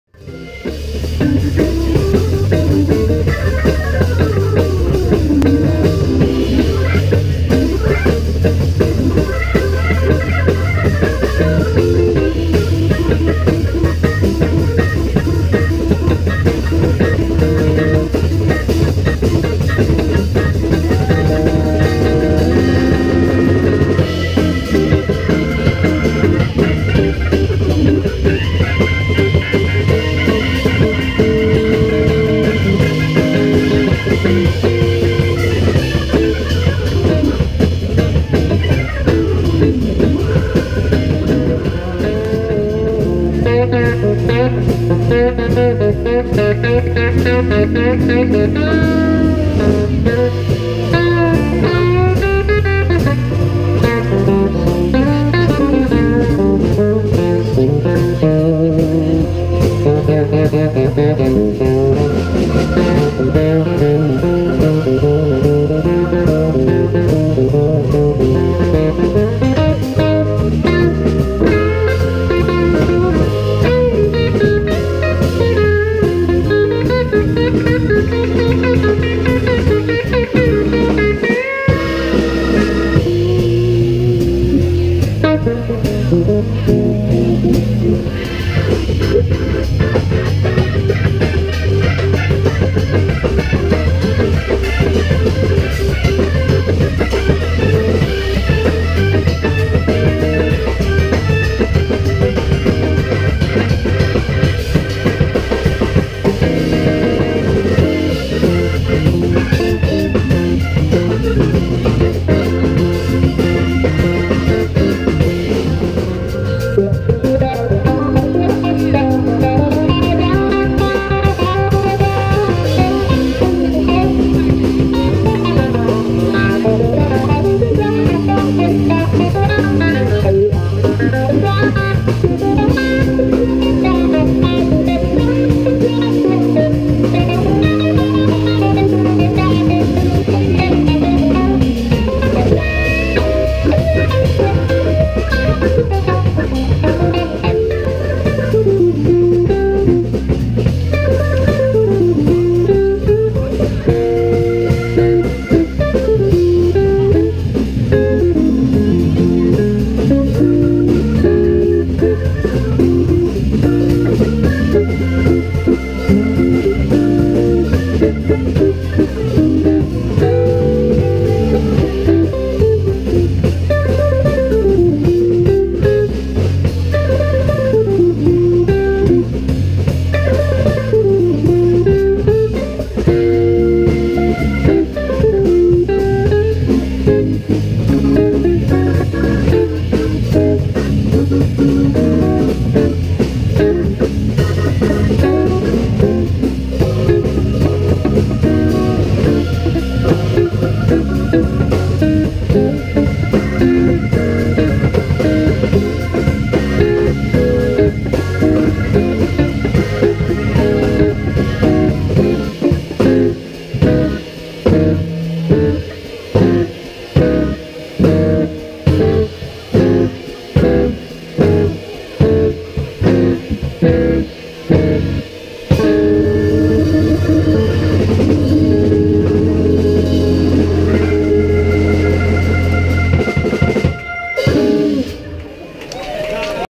Red Organ Trio EPK
The Red Organ Trio

The Red Organ Trio was born in Mississippi, steeped in the blues, and came of age in New Orleans where It now burns funk and jazz. Since forming in 2006, the trio of organist Will Thompson, guitarist Chris Alford, and an abundance of New Orleans finest drummers has married the traditional jazz organ trio and the music of New Orleans and blues pioneers alike.  It's a funky combination of Jimmy Smith, The Meters, John Lee Hooker, Dr. Lonnie Smith, James Brown, Grant Green, and Professor Longhair, to name a few.

Organ -Will Thompson
Guitar -Chris Alford
Drums -Brendan Bull